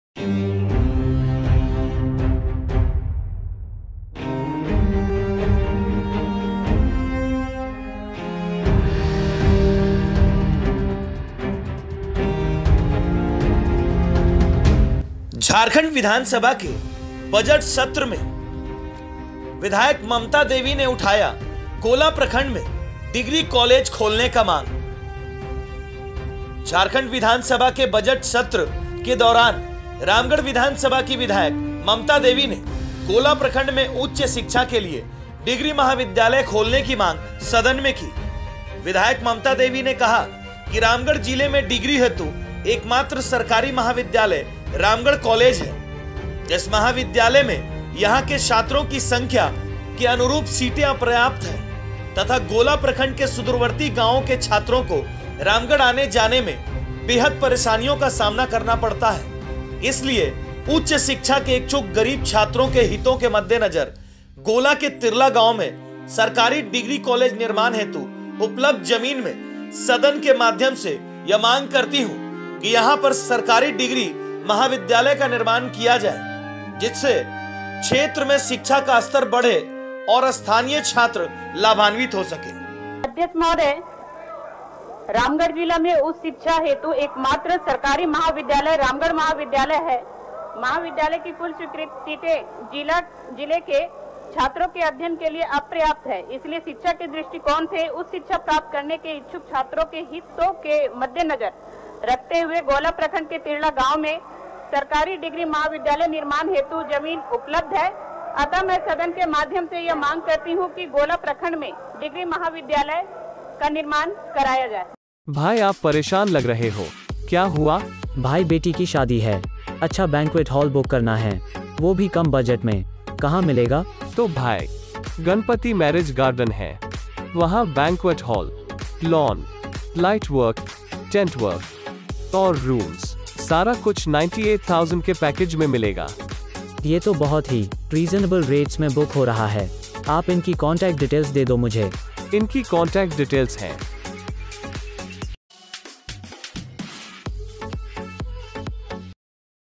विधायक ममता देवी ने गोला प्रखण्ड में डिग्री कॉलेज खोलने की मांग विधानसभा सभा में रखी
NEWS LENS

झारखंड विधान सभा के बजट सत्र के शून्यकाल के दौरान, रामगढ़ विधानसभा की विधायक ममता देवी  ने गोला प्रखंड में उच्च शिक्षा हेतु डिग्री महाविद्यालय खोलने की मांग सदन में कीं। विधायक ने कहा की रामगढ़ जिला में डिग्री हेतु एकमात्र सरकारी महाविद्यालय रामगढ़ महाविद्यालय है इस महाविद्यालय में यहां के छात्रों के संख्या के अनुरूप सीटें अपर्याप्त है तथा गोला प्रखण्ड के सुदूरवर्ती गांवो के छात्रों को रामगढ़ जाने आने में काफी परेशानी होती है,  इसलिए उच्च शिक्षा के इच्छुक गरीब छात्रों के हितों के मद्देनजर गोला के तिरला गांव में सरकारी डिग्री कॉलेज निर्माण हेतु  उपलब्ध जमीन में सदन के माध्यम से यह मांग करती हूं कि यहां पर सरकारी डिग्री महाविद्यालय का निर्माण किया जाए जिससे क्षेत्र में शिक्षा का स्तर बढ़ने और स्थानीय छात्र लाभान्वित हो सके।